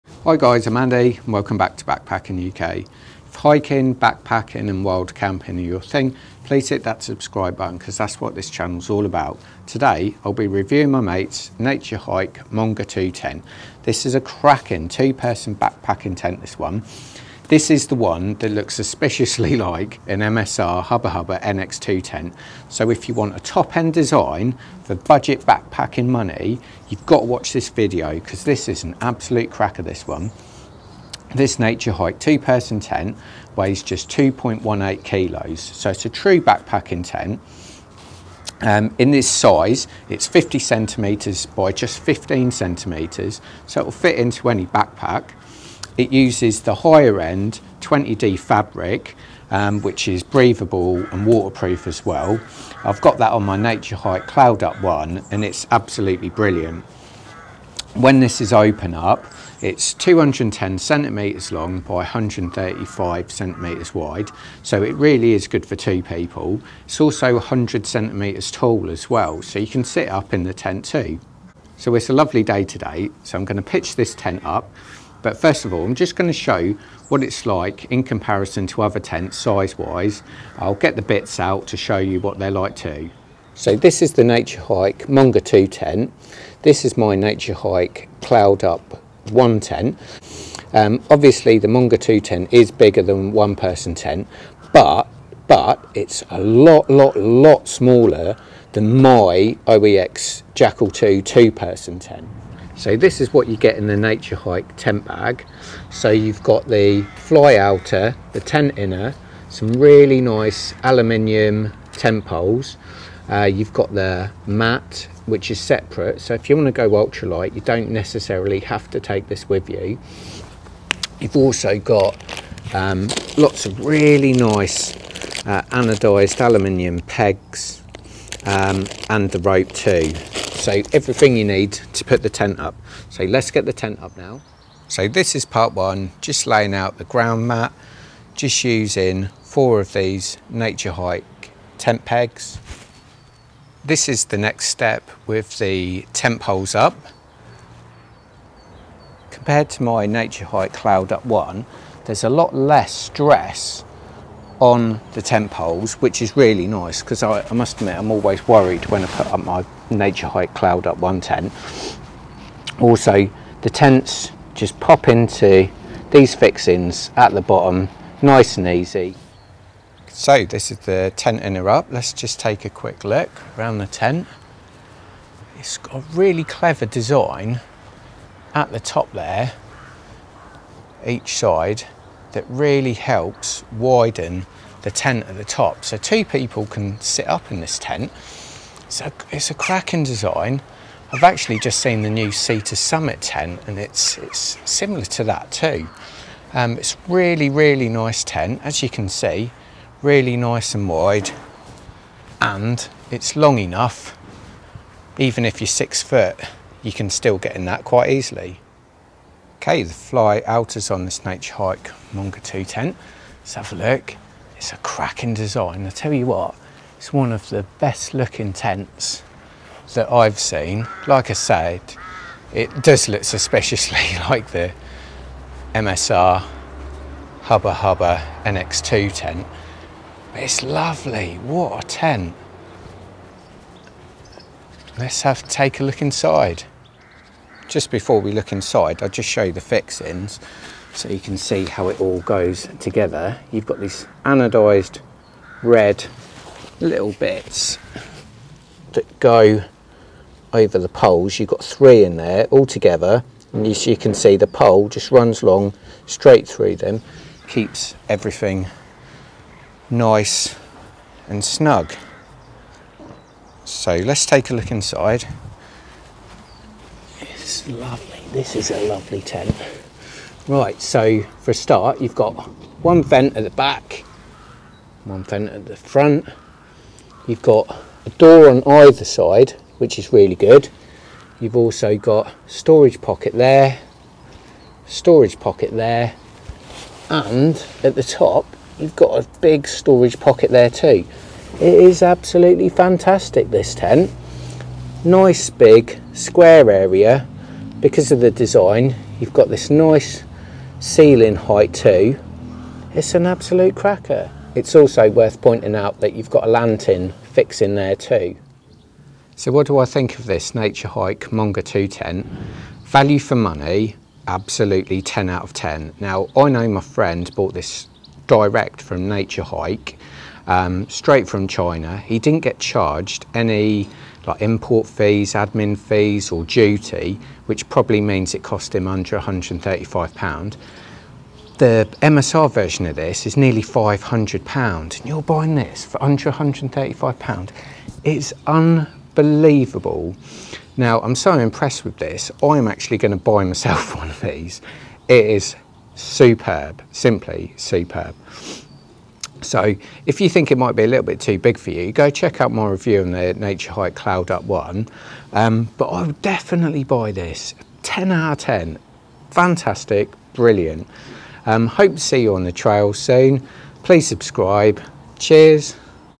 Naturehike Mongar 2 Tent - The BEST 2 Person 3 Season BUDGET Tent?
Here is my Naturehike Mongar 2 Tent Review. This is a superb two person tent perfect for backpacking and wild camping. At 2.18kg it is a true backpacking tent. This spacious 2 person tent would be ideal for two people to take away with them wild camping in the uk.

At under £135 this wild camping tent is superb value for money and a true piece of budget backpacking gear.  This really could be the best budget tent.

or £155 from Amazon

This two person tent is spacious at 210cm long x 135cm wide and 100cm tall. It also has a door on either side of the tent.

This camping tent is packed full of features including two vents, two storage pockets, one large storage net at the top and a hook for a lantern. The 20D fabric is strong and light as well as being waterproof.

Naturehike tents are fantastic and I'm really happy with my Cloud Up 1 tent. However, this Mongar tent is next level.

on the Amazon Prime Naturehike UK Store.

The Mongar tent is definitely much more futuristic than than quite ordinary Naturehike Cloud Up 1 tent. The Mongar tent has some great design features  as seen on much more expensive wild camping tents.

Thanks, Andy.

00:00 Introduction & Specification
01:23 What's Included
02:15 Pitching
03:29 Features
04:23 Take a Look Inside
05:09 Summary

Please note the link is what everyone sees and pays but I receive a miniscule finders fee when people use the link. It all goes back into backpacking gear!